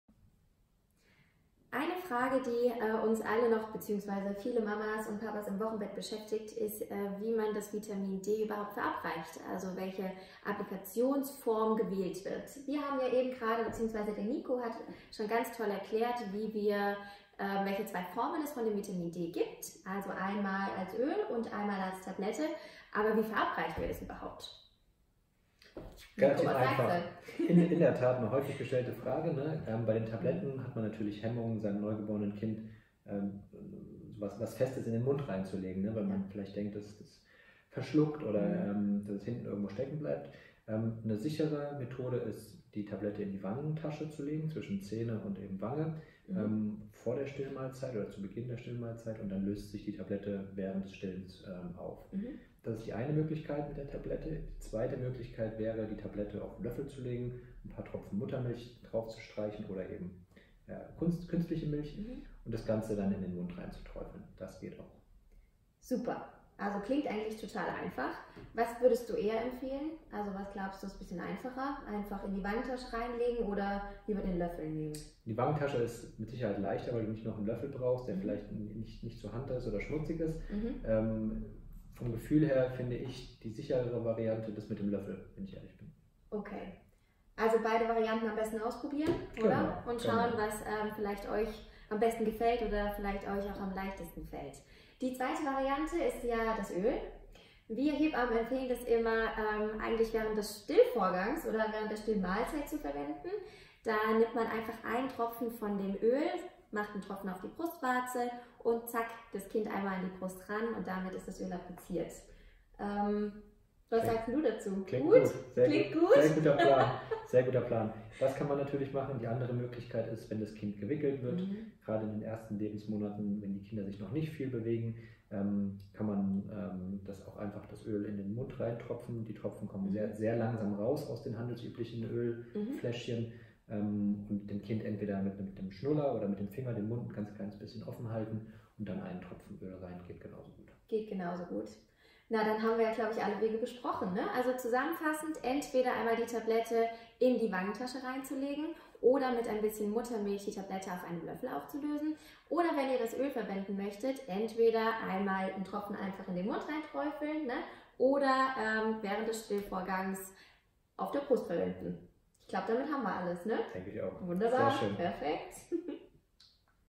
Wie gibst du deinem Kind Vitamin D?
In diesem Video spricht Hebamme Marie von hallohebamme mit dem Kindarzt Dr. Nikolai Holm-Hadulla darüber, wie du Vitamin D am einfachsten deinem Kind verabreichen kannst.